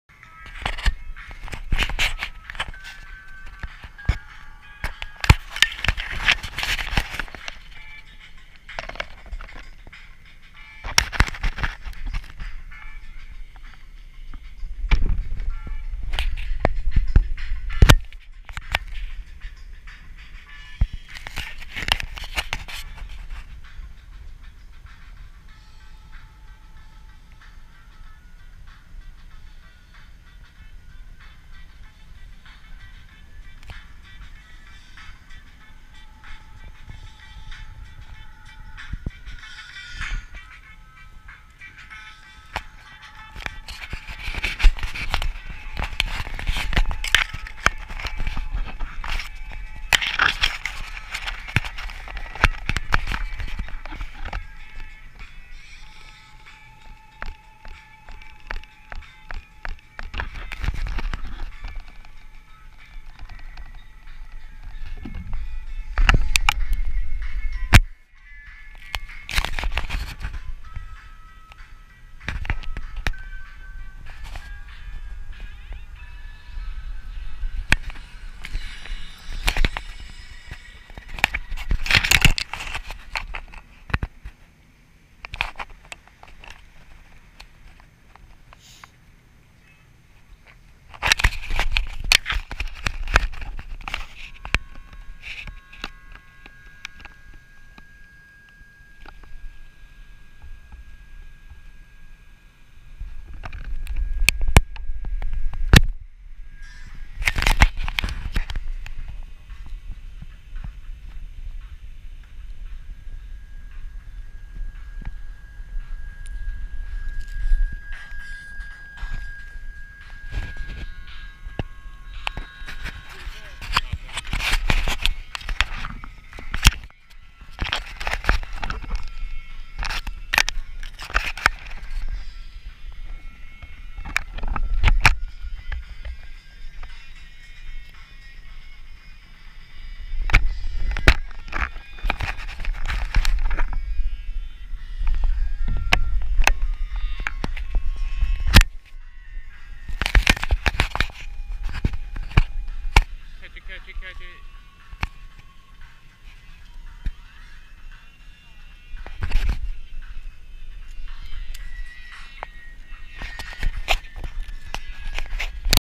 Cometcopters LED Copters
Brandon flying Cometcopters LED Copters that are amazing fun.